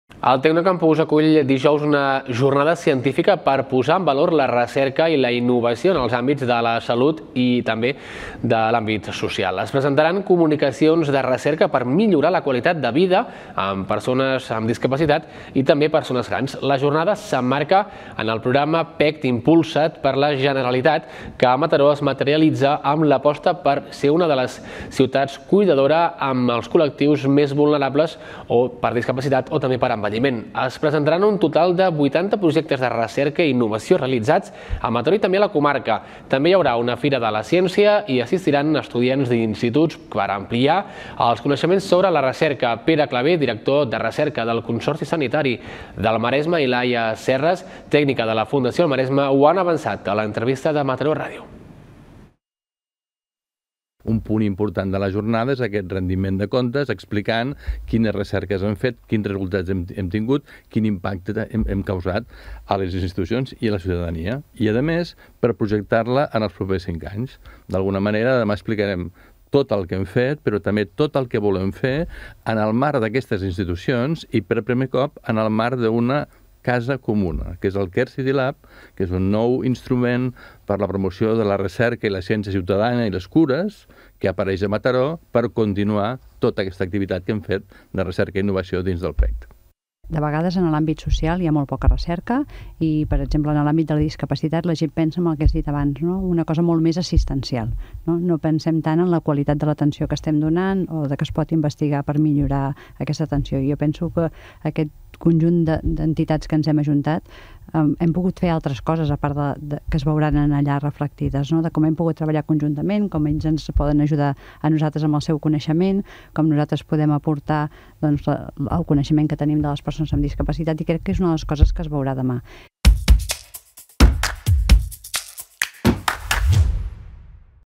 Jornada per posar en valor la recerca en els àmbits social i de salut
El Tencnocampus acull dijous la Jornada Ecosistema de de les Ciutats Cuidadores. Pere Clavé, director de recerca del CSdM i Laia Serras, de la Fundació Maresme, expliquen a l'Entrevista de Mataró Ràdio que 'es donaran a conèixer 80 projectes de recerca'